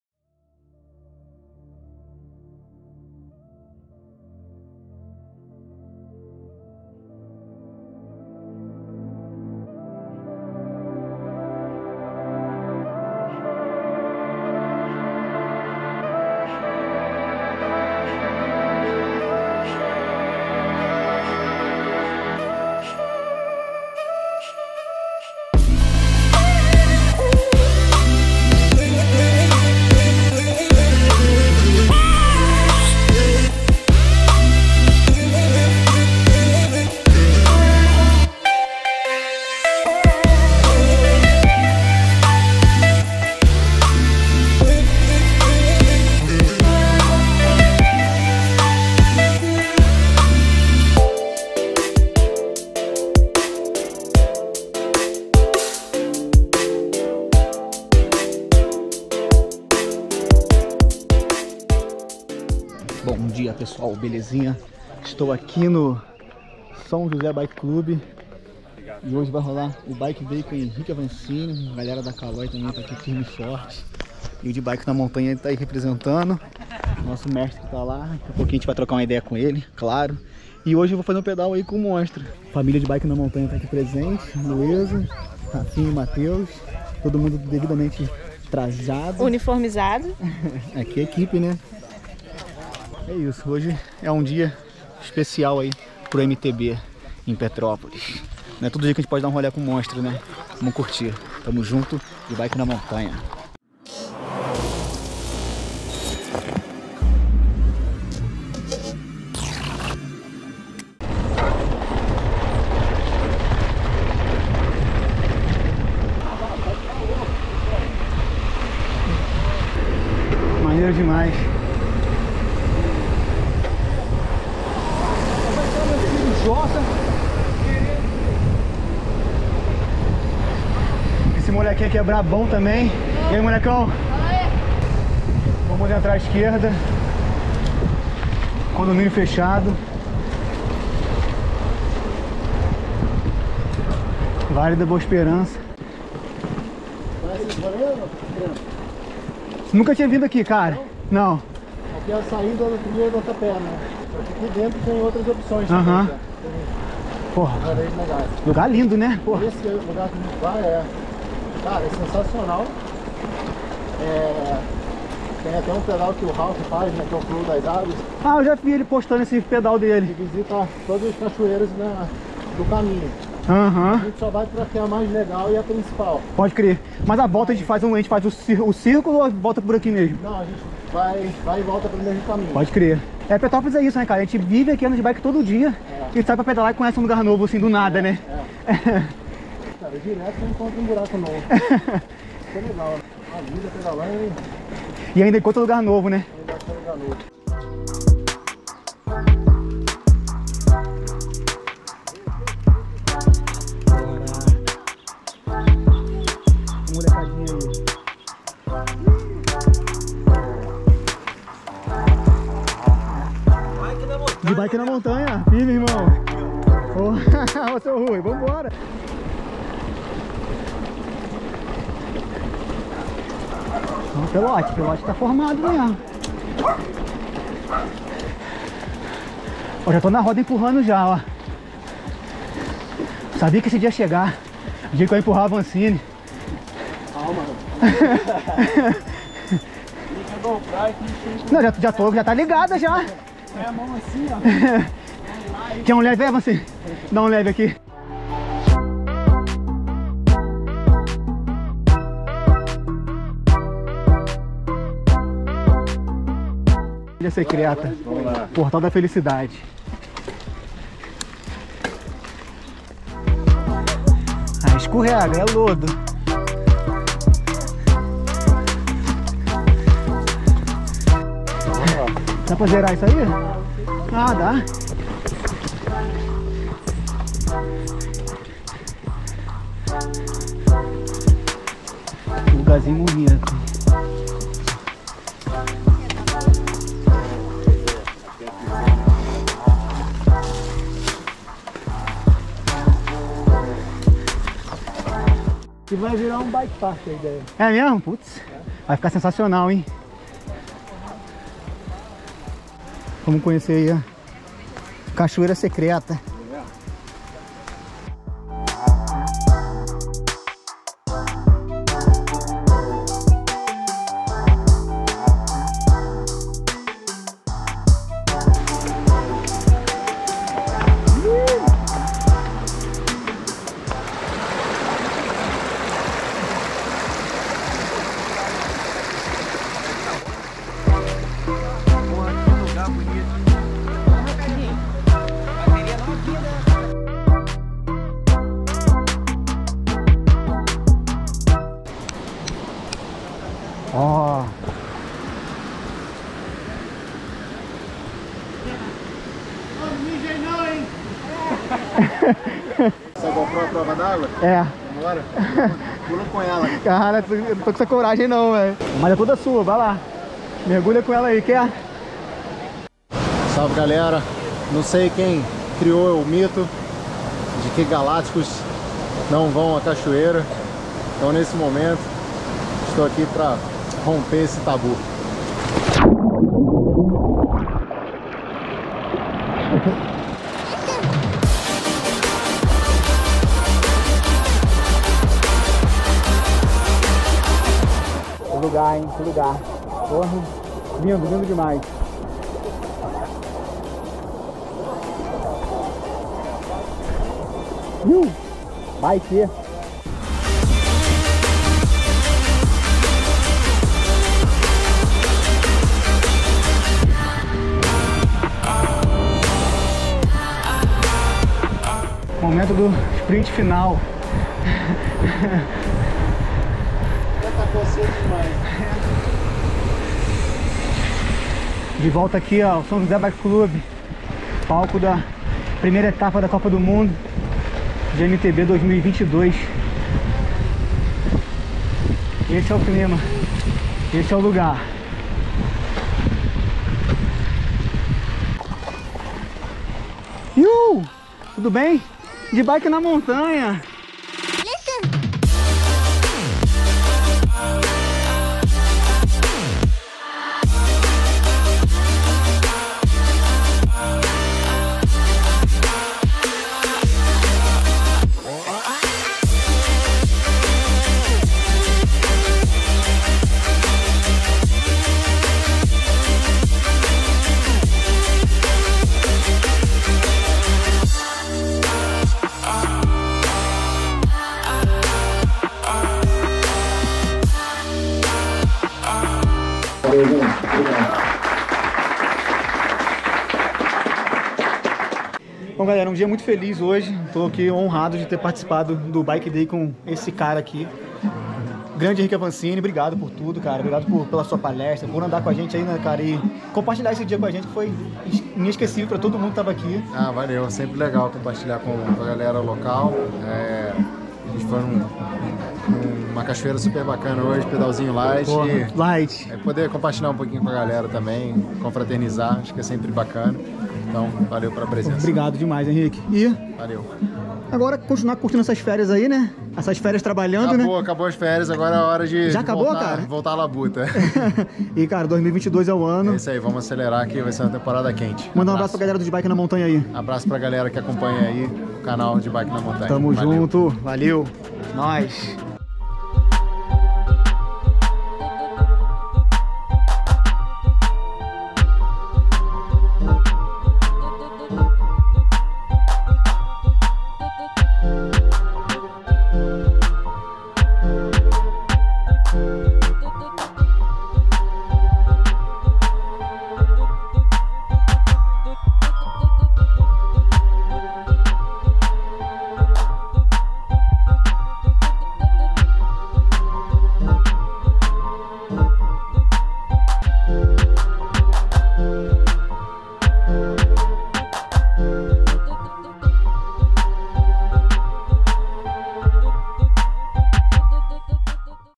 BIKE DAY COM HENRIQUE AVANCINI
Bike day com o nosso campeão @avancinimtb 🏆

Geralmente nos finais de ano ele faz uma pedal de confraternização com amigos e clientes da loja Avancini Bikes e tive a honra de ser convidado e claro que aproveitei pra registrar um pouco desse dia incrível. Além do pedal, depois rolou um bate papo muito maneiro com o Henrique onde ele falou do ano que passou e do futuro em cima da bike!

📸 Nosso Insta: @debikenamontanha
⚡️ Strava: Bruno Canella